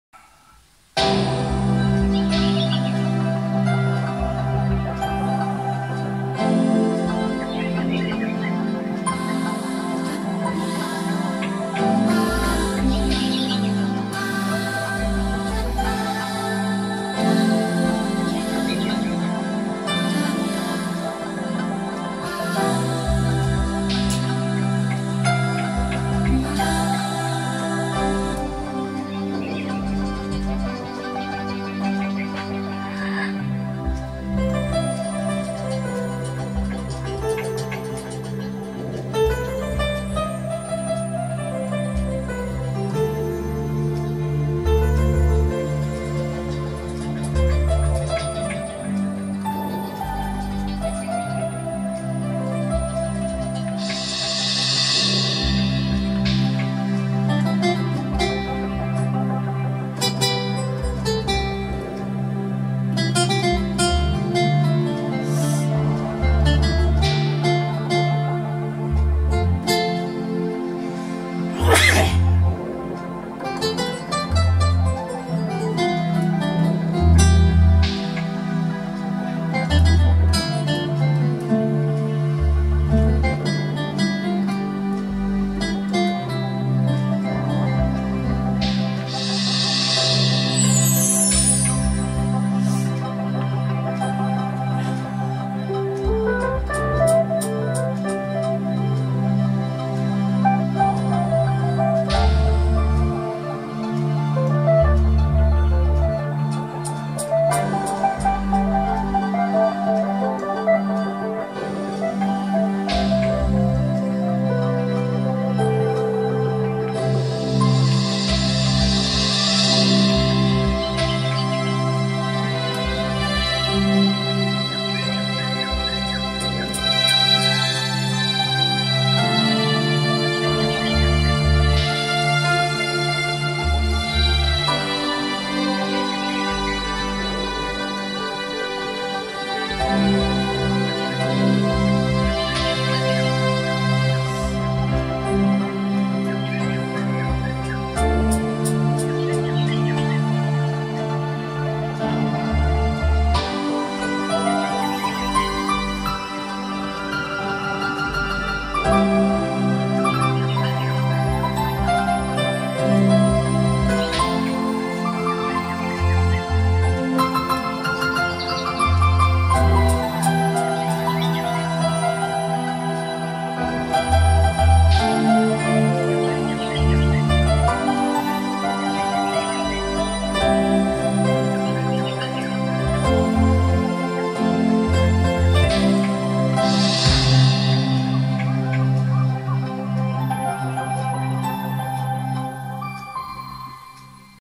respire
je joue avec mon pa5x et mon genos